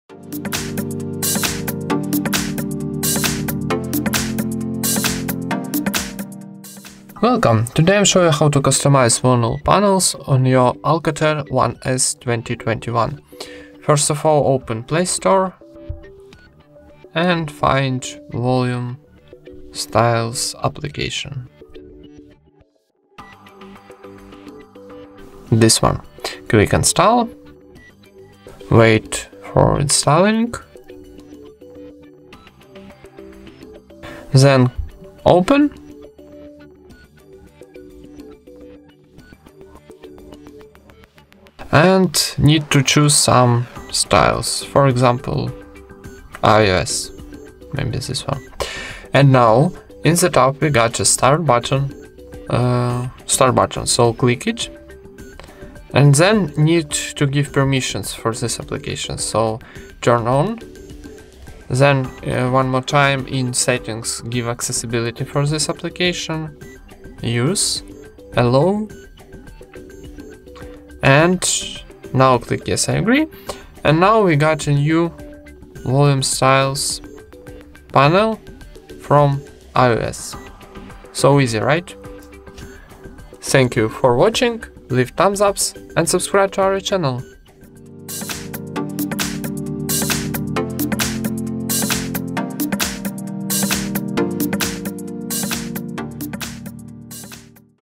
How to Customize Volume Panel in ALCATEL 1S 2021 – Volume Styles App
Did you know you can change the volume panel on your ALCATEL 1S 2021? If you want to set the volume panel like in iPhones, open this tutorial and learn how to download and install the Volume Styles App. Find out how to apply the new style volume panel on your ALCATEL 1S 2021. Do you want to customize every detail in your smartphone? Go to our YouTube channel and use tutorials created by our specialists.